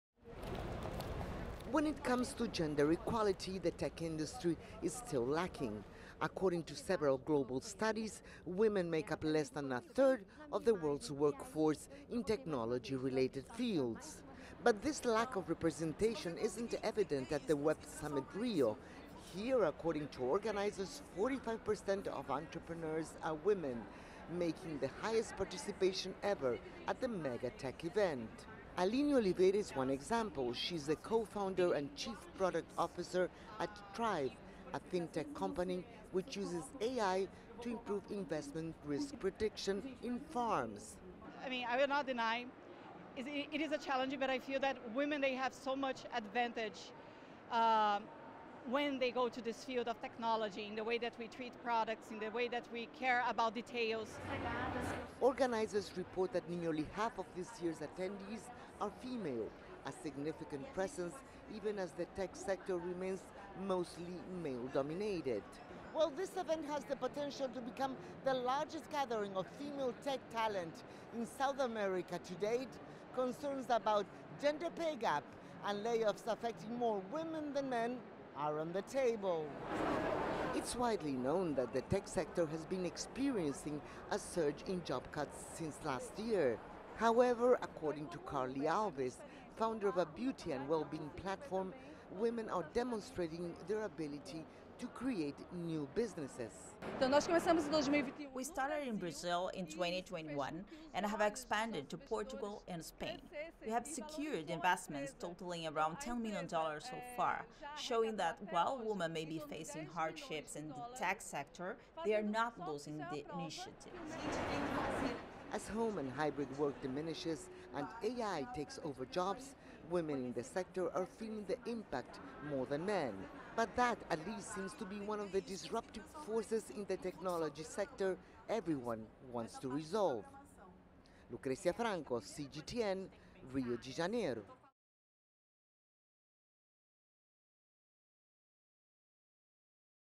Women-founded startups take the spotlight at Web Summit Rio
The world's largest conference on technology and innovation is held in Rio de Janeiro. The event, Web Summit Rio, has broken records when it comes to female participation among entrepreneurs, attendees, and speakers. The Women in Tech Lounge is an exclusive area aimed at empowering women in the world of digital technology. Check it out.